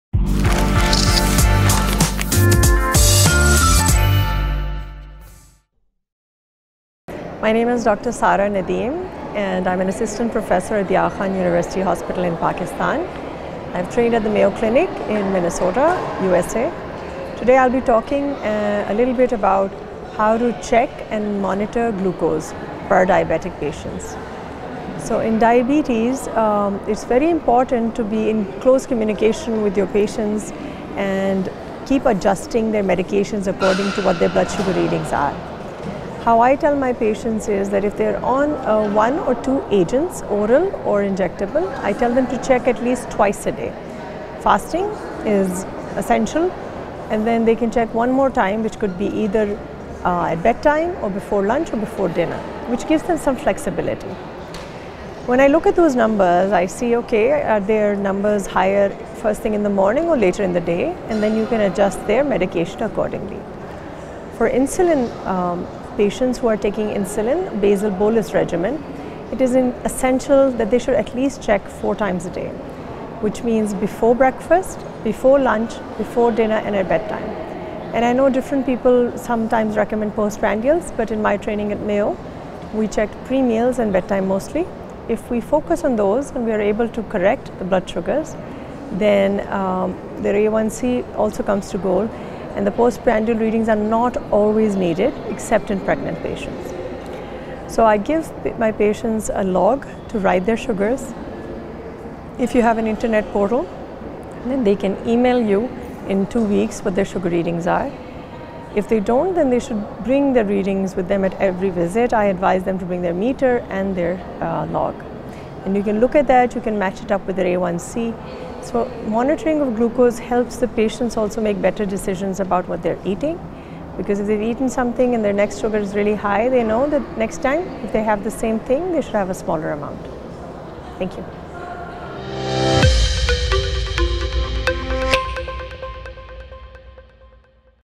Prof. Sarah Nadeem talks about how to Monitor glycemic for diabetic patients
Prof. Sarah Nadeem
Assistant Professor of Endocrinology medicine at the Aga khan university